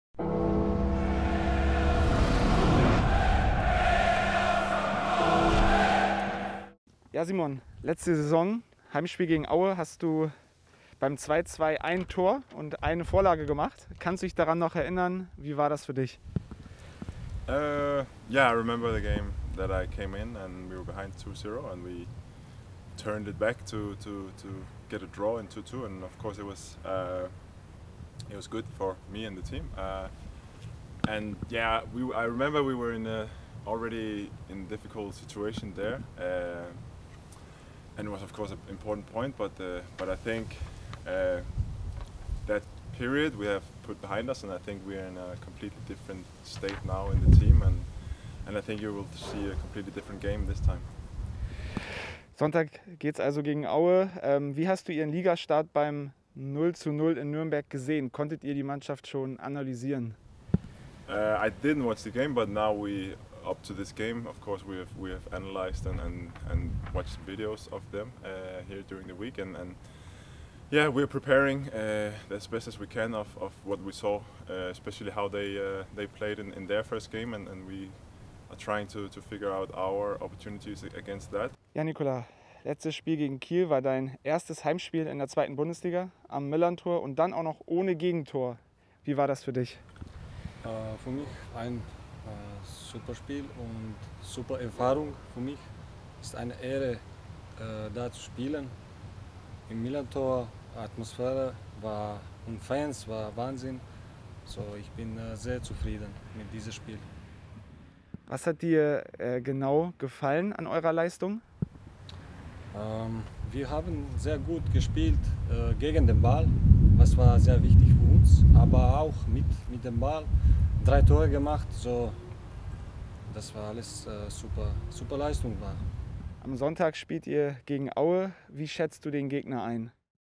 Simon Makienok und Nikola Vasilj vor dem Spiel bei Erzgebirge Aue
Wir haben vor dem Spiel bei Erzgebirge Aue mit Simon Makienok und Nikola Vasilj gesprochen.